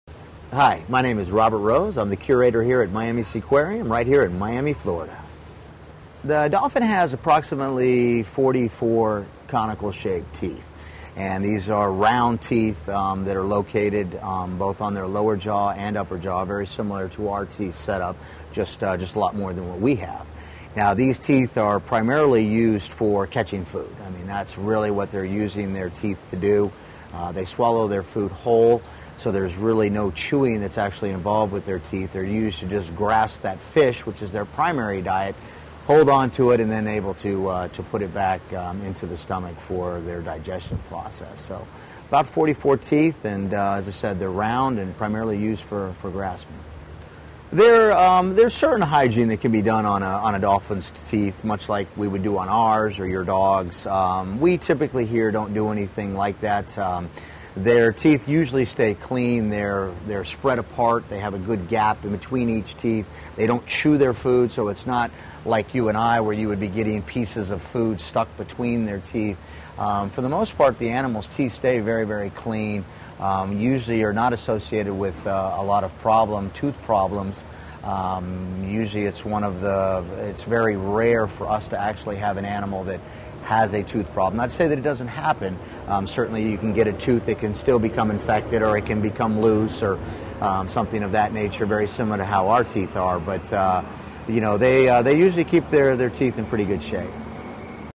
How Many Teeth Does a Dolphin Have?
How Many Teeth Does a Dolphin Have?. Part of the series: Facts About Dolphins. A dolphin has approximately 44 conical-shaped teeth that are located on both their upper and lower jaw. Learn more about dolphin teeth, which are primarily used for catching fish, with help from an aquarium's curator in this free video on facts about dolphins.